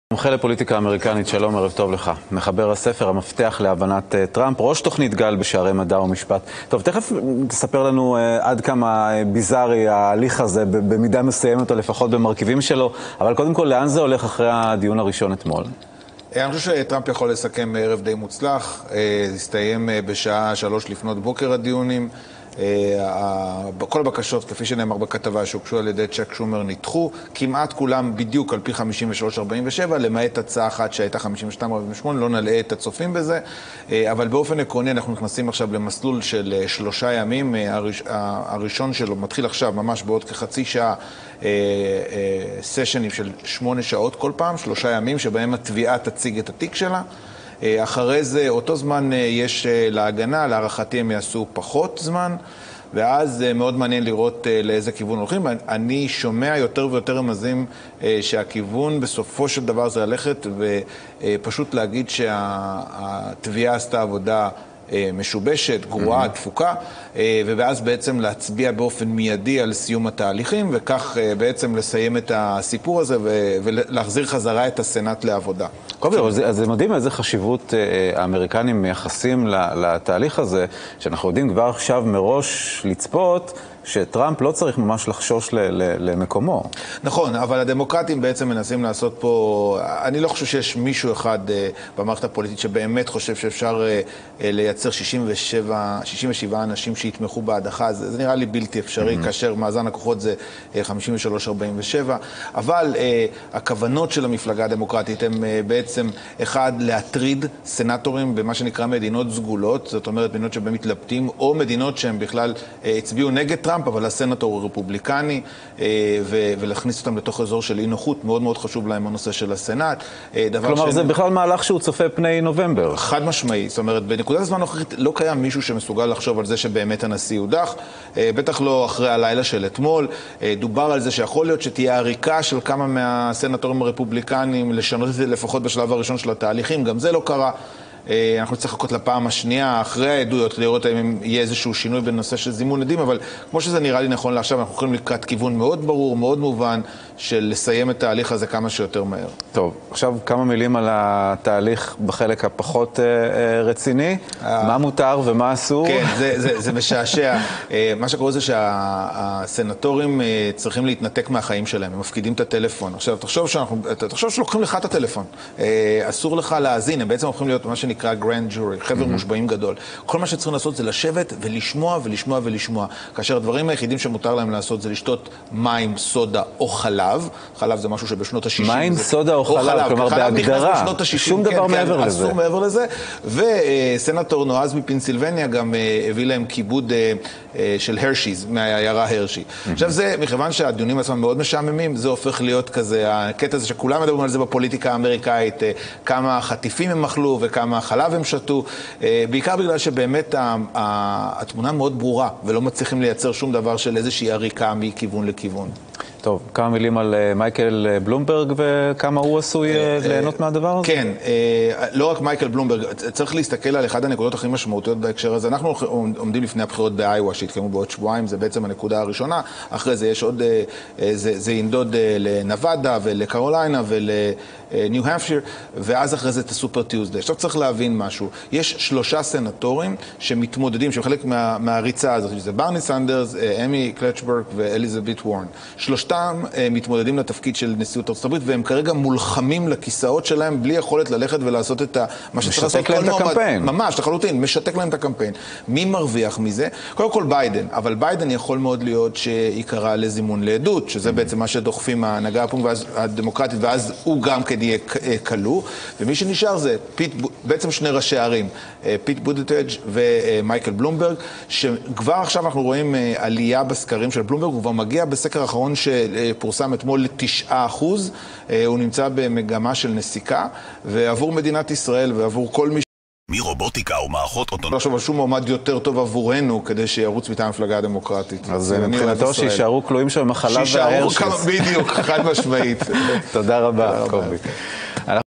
על תחילת משפט ההדחה בסנאט impeachment proceedings in the Senate
I was interviewed yesterday about the impeachment proceedings in the Senate and what will happen at the end of Channel 20 which is the Israeli "Fox". Click on subtitles to receive the English translation
ראיון בערוץ 20 על התחלת הליך ההדחה נגד הנשיא דונאלד טראמפ, בדיוק שלוש שנים לאחר השבעתו לתפקיד